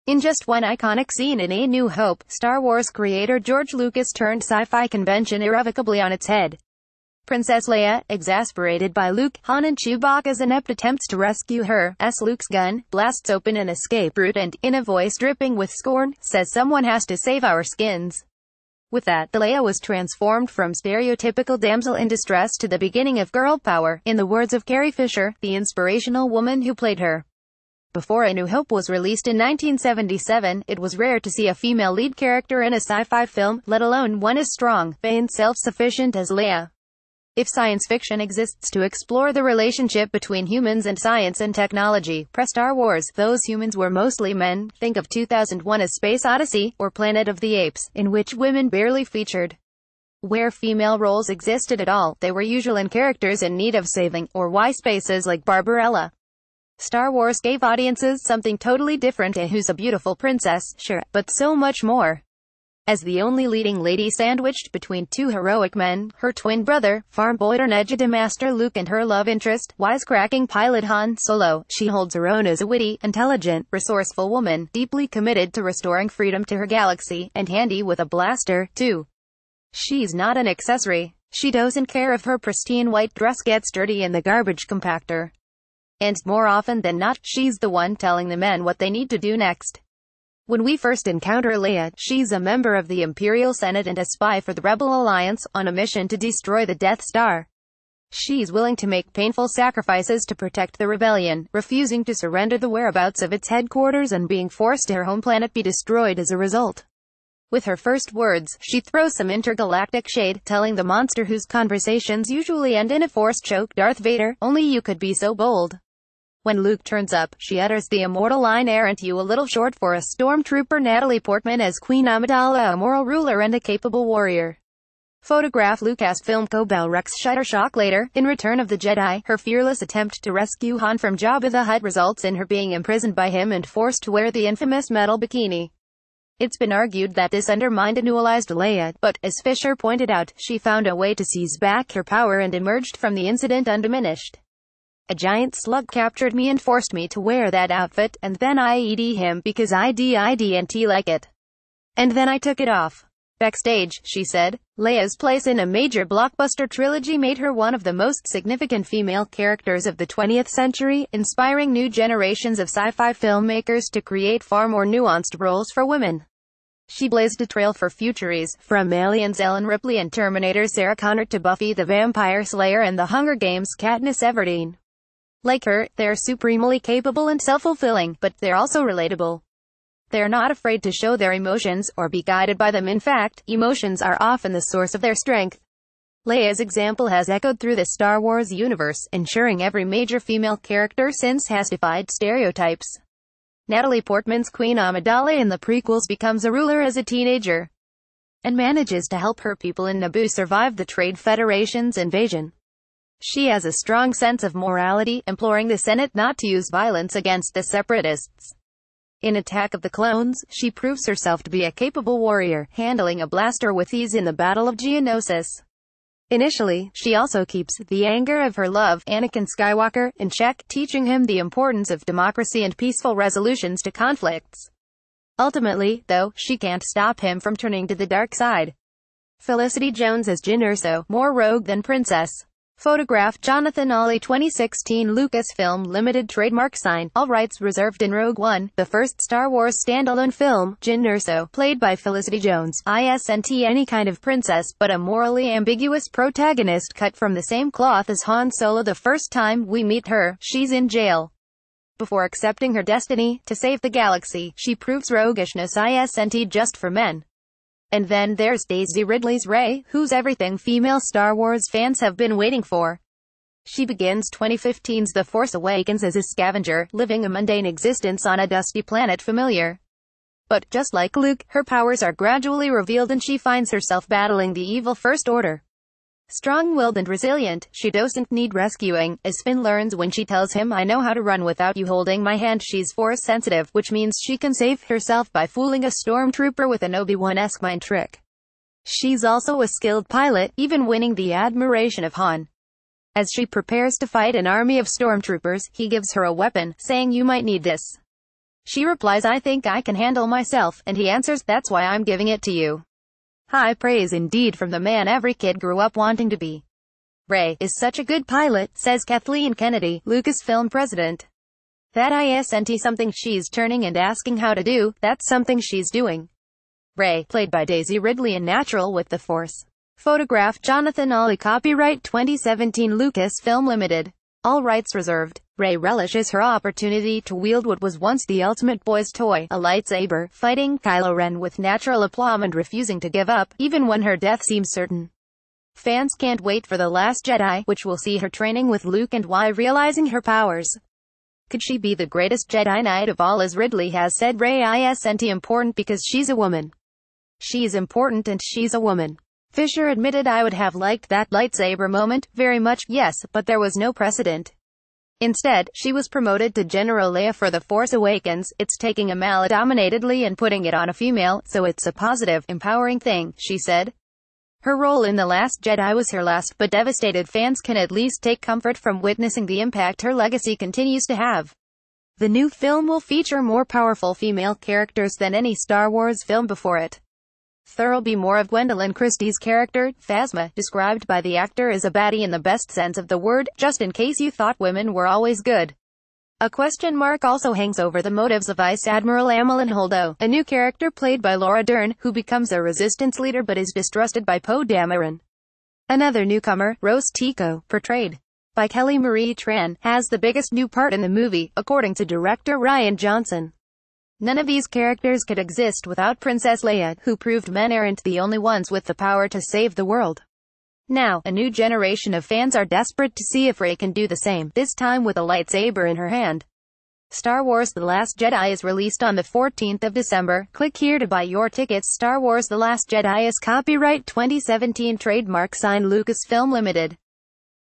Don’t mess with a princess: how the women of star wars changed sci-fi forever
In just one iconic scene in A New Hope, Star Wars creator George Lucas turned scifi convention irrev...